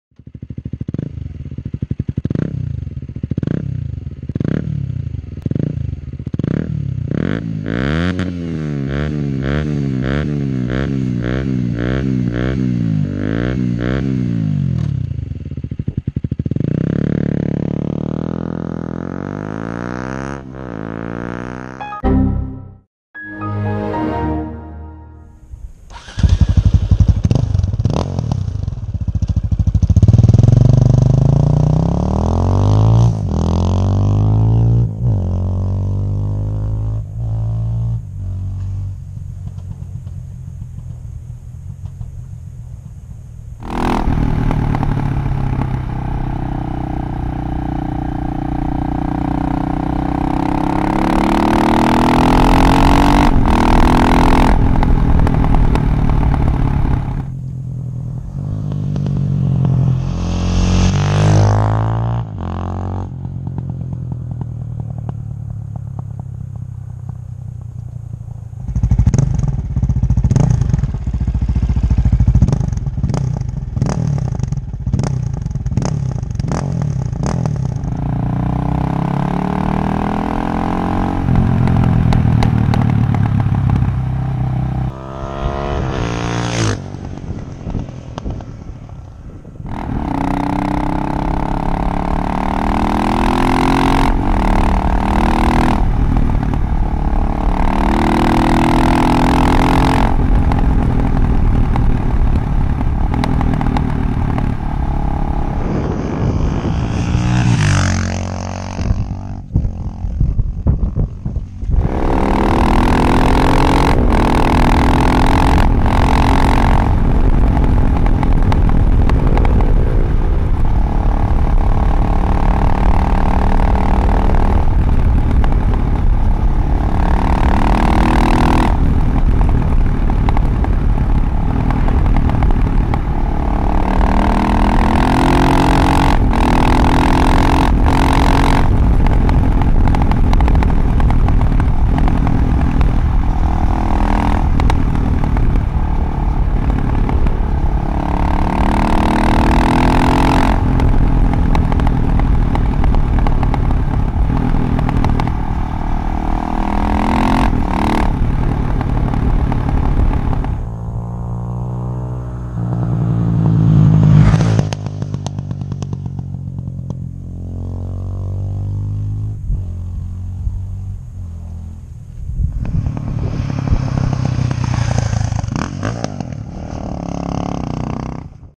The sound of burning fuel | Soundcheck KTM LC4 640 Duke II
Soundcheck of my KTM Duke ´02